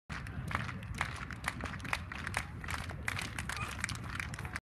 Discus throw! 56.26m (1.75kg) Thomas Mardal
New PB and gold in the Norwegian junior championship